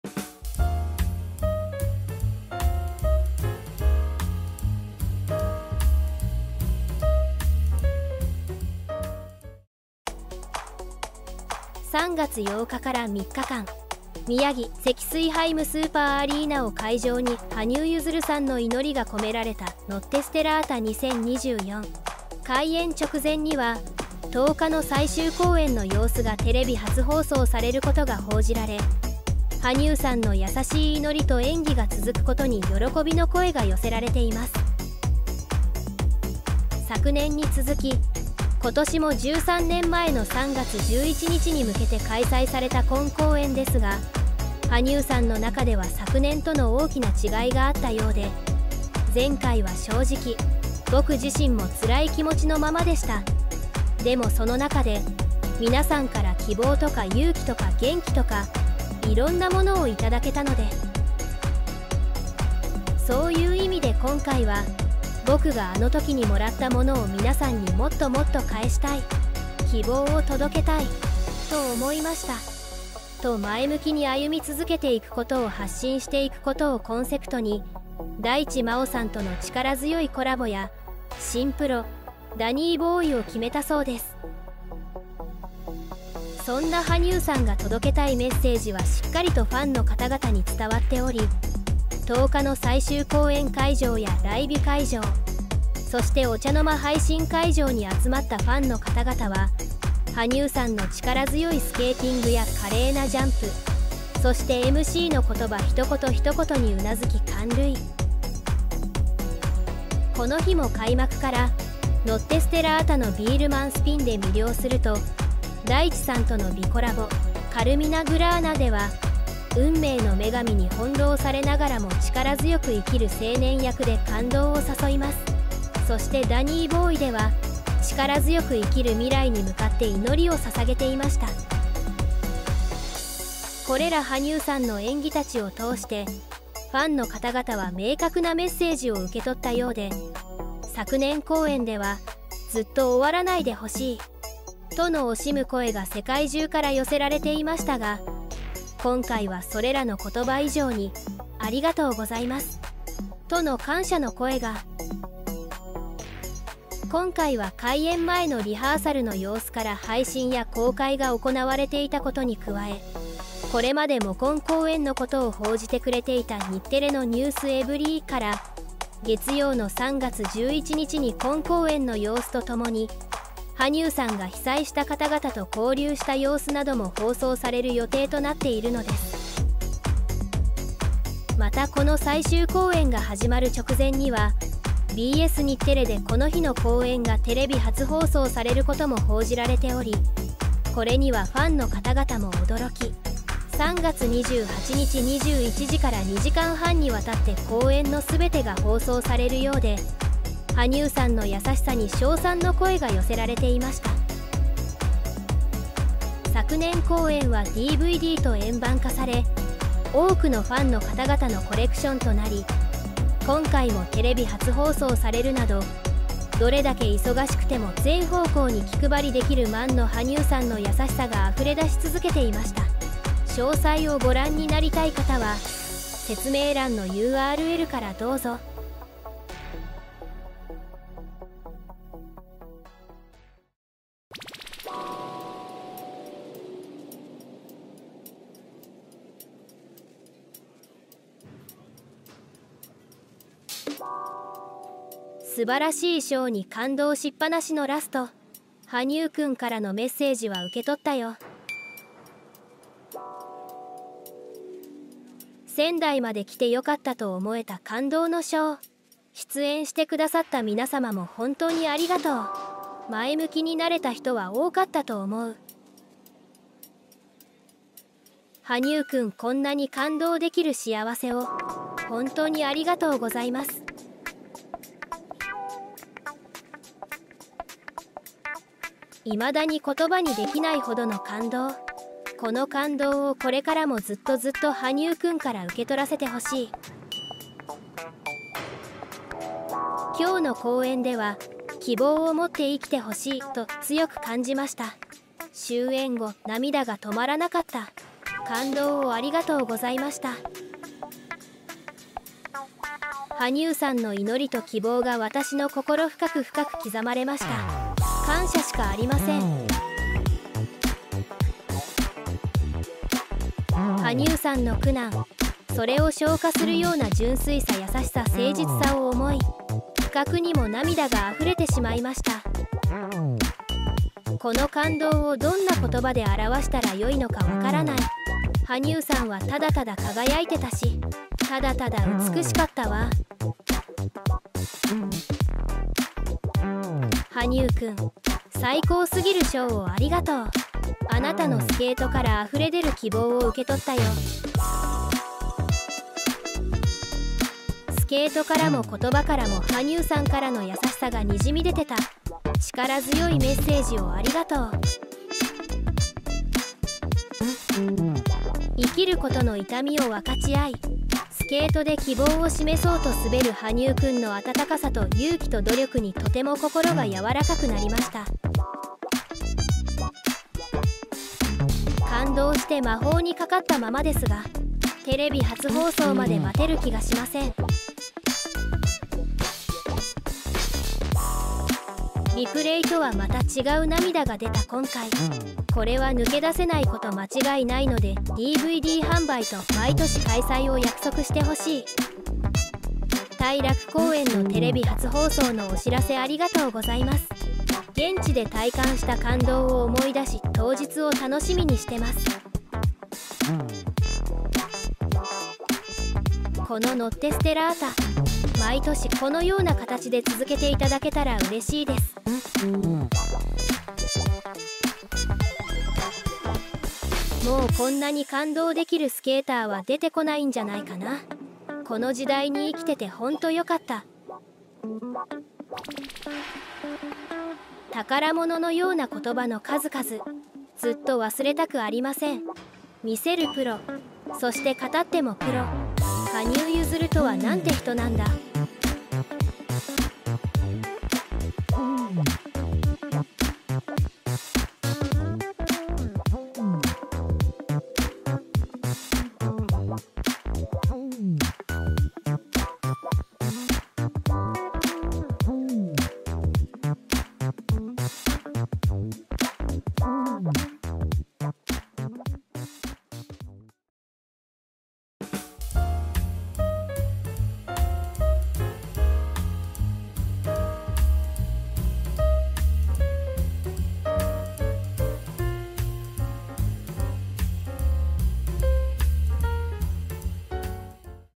【notte stellata 2024 最終日】「この感動が今度はテレビで⁉︎」羽生さんの優しい祈りと卓越しすぎた演技に感涙…❤︎
3月8日から3日間、宮城・セキスイハイムスーパーアリーナを会場に羽生結弦さんの祈りが込められたnotte stellata 2024❤︎開演直前には、最終日となった10日公演の様子がテレビ初放送されることが報じられ、羽生さんの優しい祈りと演技が続くことに喜びの声が…♪

■BS日テレでテレビ初放送決定！（PR TIMES・公式SNS）

★ブログ！

★アンケート！